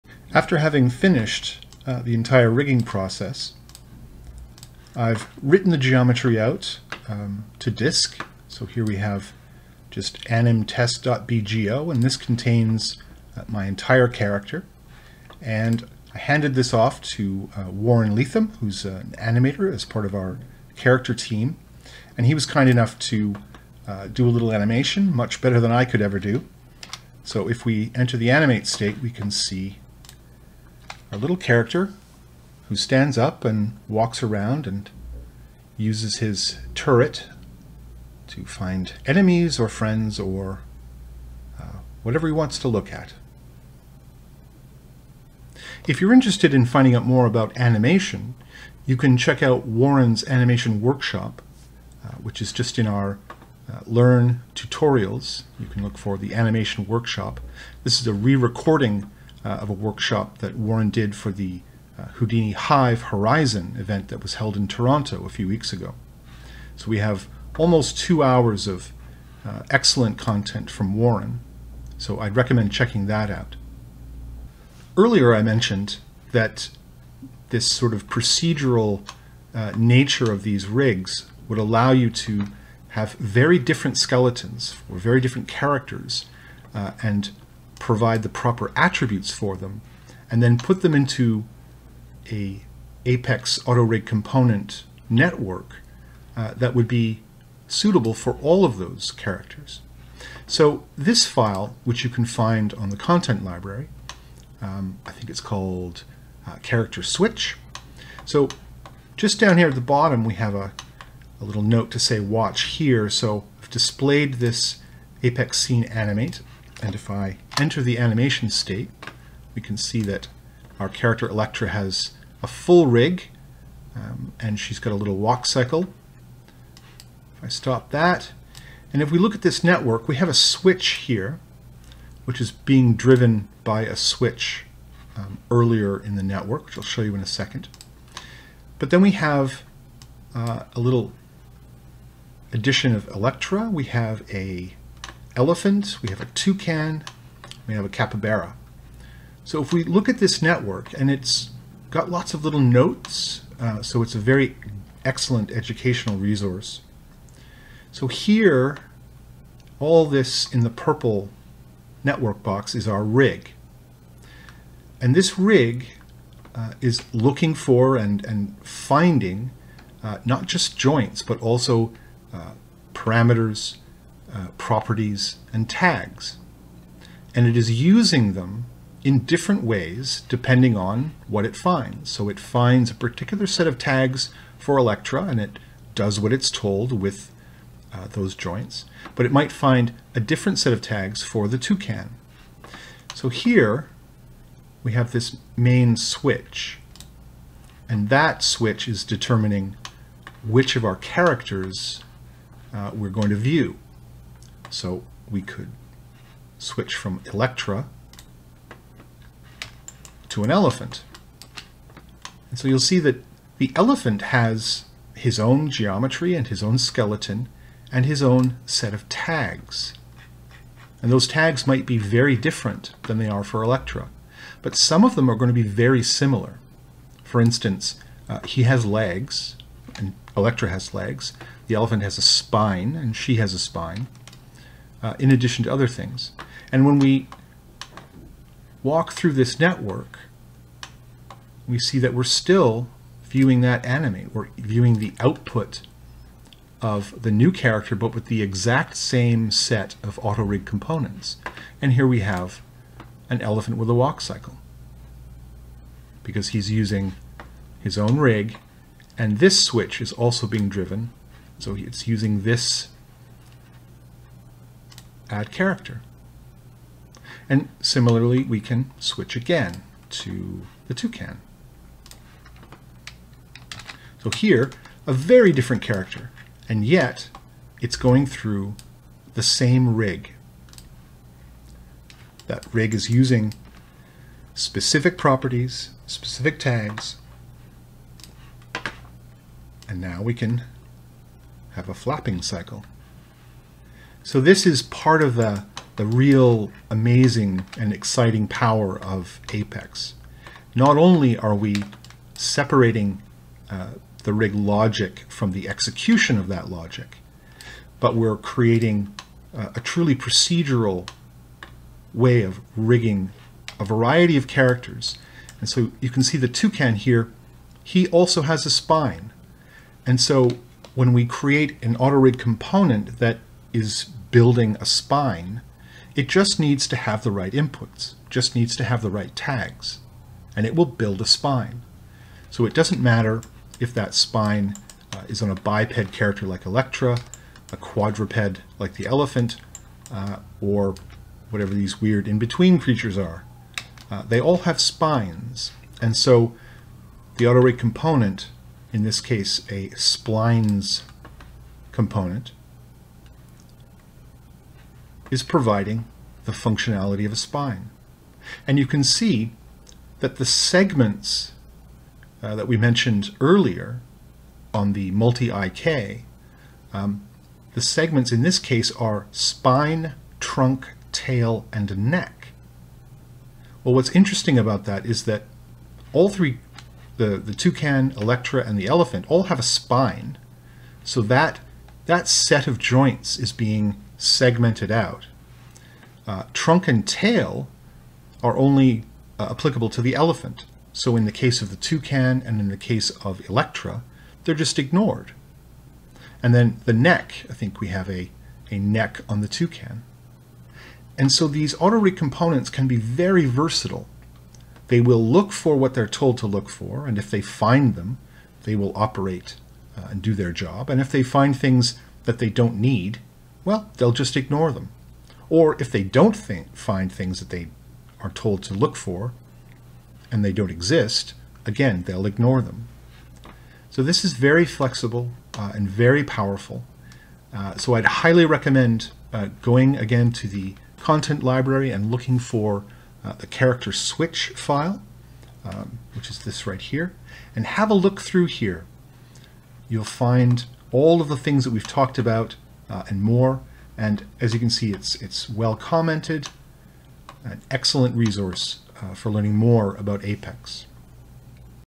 KineFX Rigging with APEX 9 | Final Rig and Resources
Finish the rig and take a look at some additional resources for learning more about rigging in Houdini with APEX.